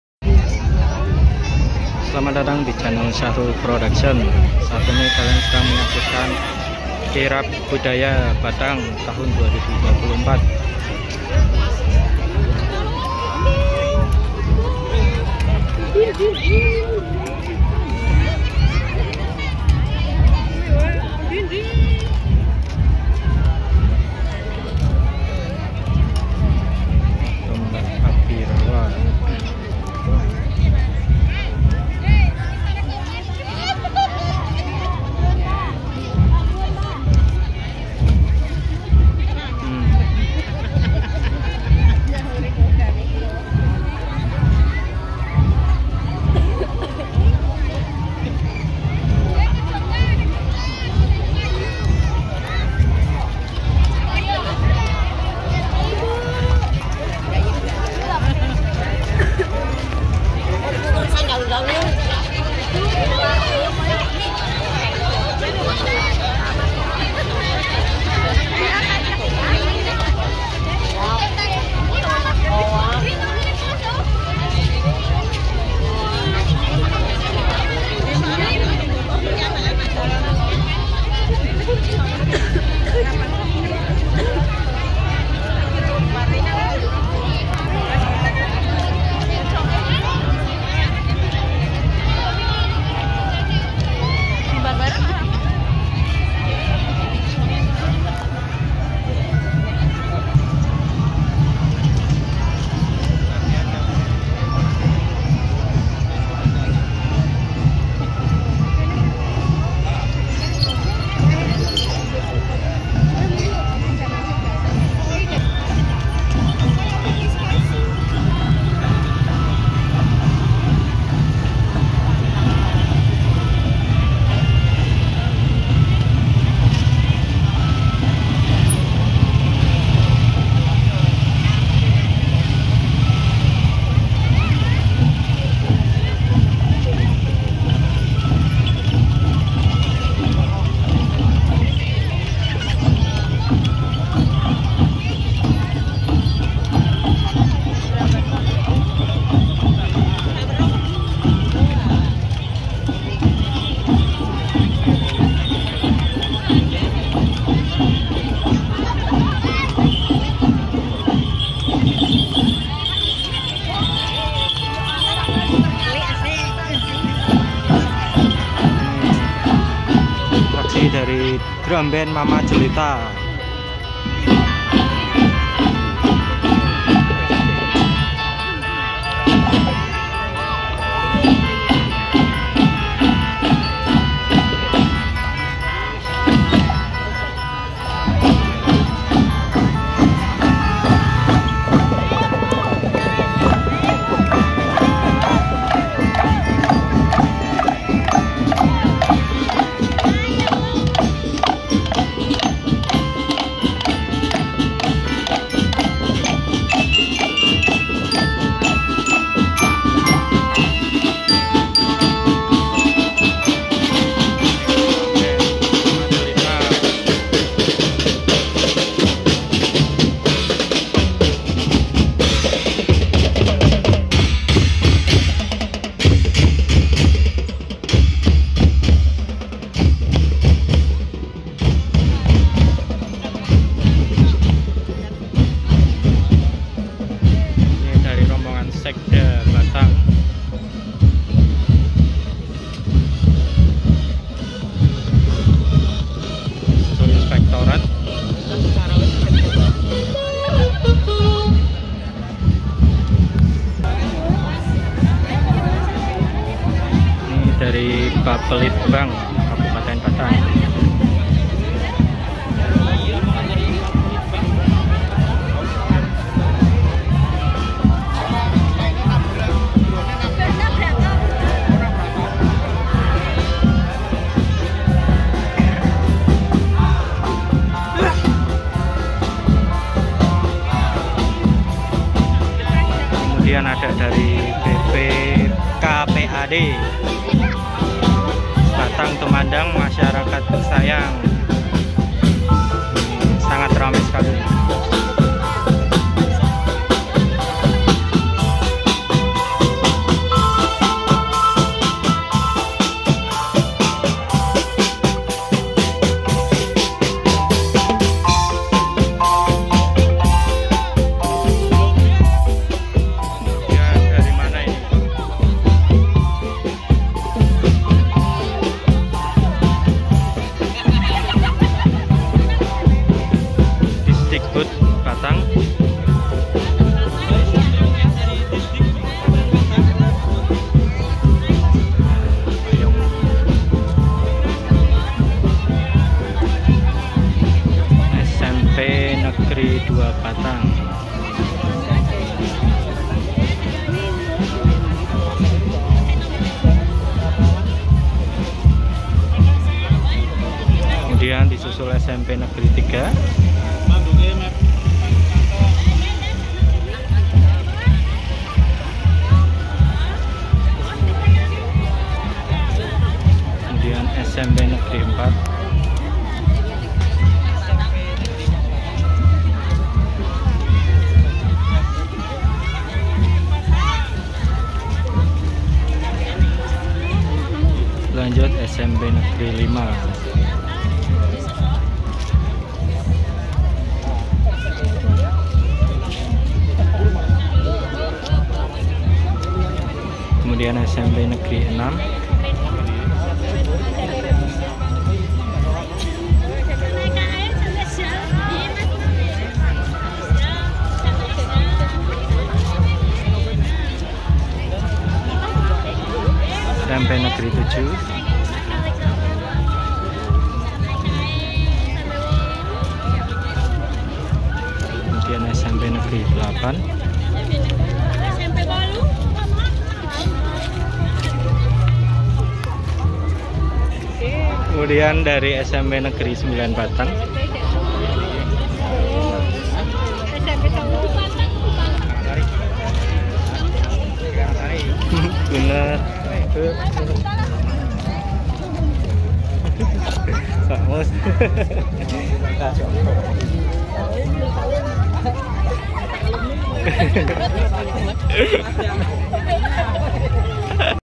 RAMAINYA KIRAB BUDAYA HUT KABUPATEN BATANG TAHUN 2024 || TOMBAK ABIRAWA - PASUKAN SMP BATANG
RAMAINYA KIRAB BUDAYA HUT KABUPATEN BATANG TAHUN 2024 - DRUMBAND MAMA JELITA BERAKSI